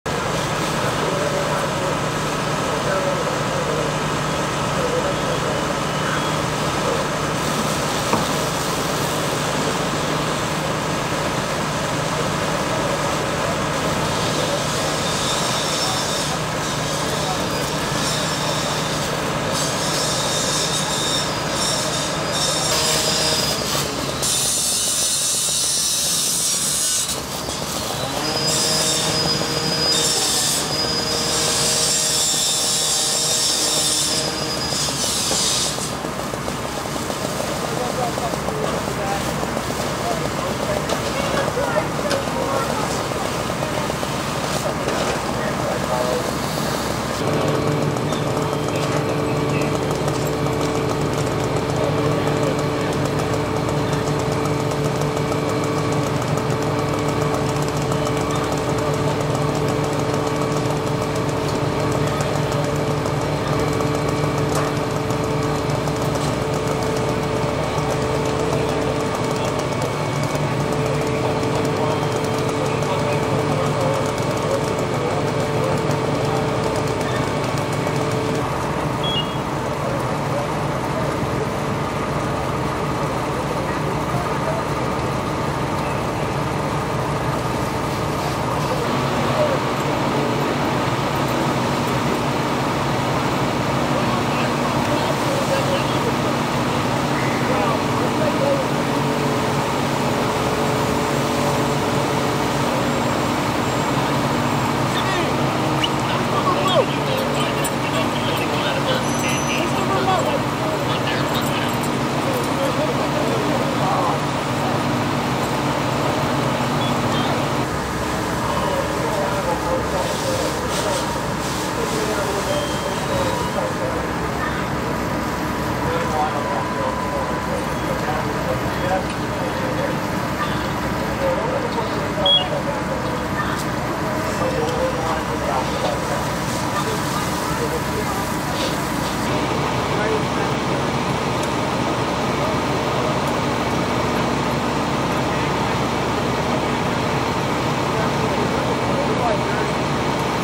Fully Involved House Fire / Upper Macungie, PA 2.11.23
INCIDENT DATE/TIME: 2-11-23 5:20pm
LOCATION: 8583 pathfinder rd
CITY: upper macungie
DETAILS: 1 home fully involved 2 more home damaged by fire no inj reported

Twitter LLN PHILLY